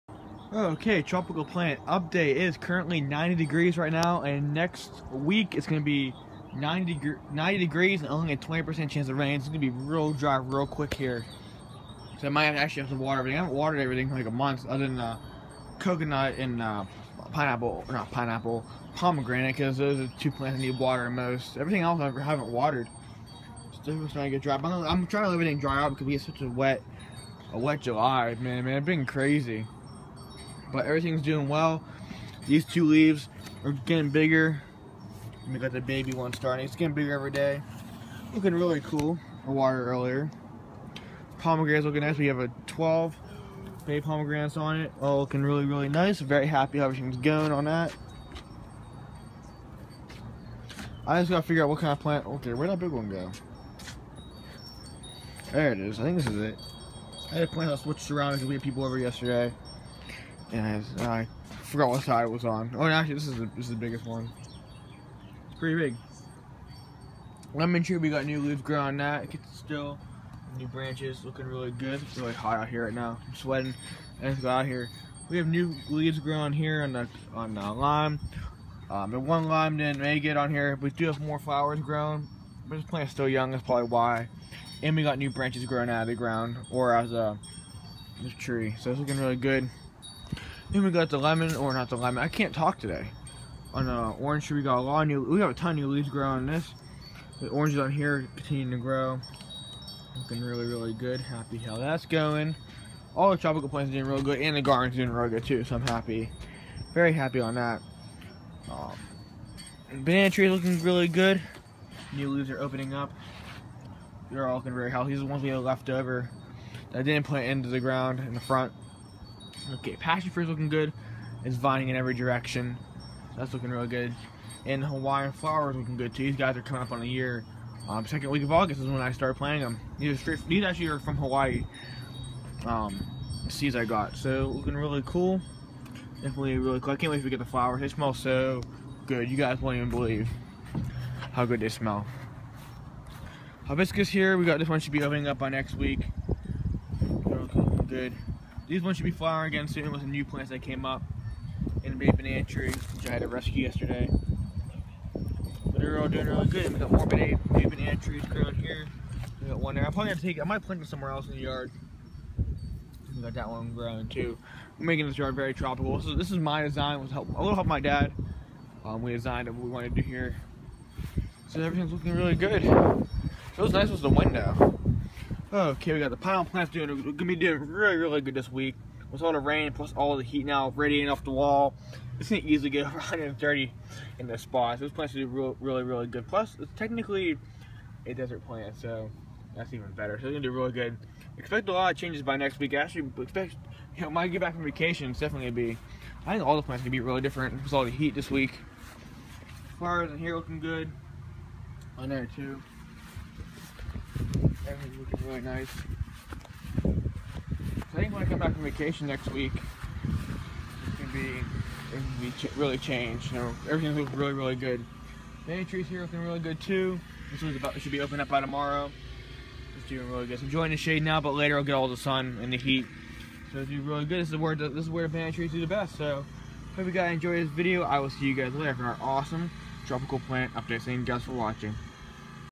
Tropical Plant 2015 Daily UPDATES: (12 BABY FRUIT!) Day 76
I will be doing videos EVERYDAY on my tropical plants!!! It’s going to be a lot of fun for sure!!!
I live in Pittsburgh PA and somehow manage to have tropical plants that should only live in the Deep South or in tropical parts of the world.  Early summer it gets into the 70-80 with a lot of rain, by July it’s very hot and dry, 80-95 degrees.
The plants I have:
• Pomegranate
• Bananas
• Orange
• Lime
• Lemon
• Pineapple
• Fig
• Mango
• And yes even a Coconut tree in Pittsburgh!!
It’s going to a fun summer watching all these grow!! If you missed any days Click Here!!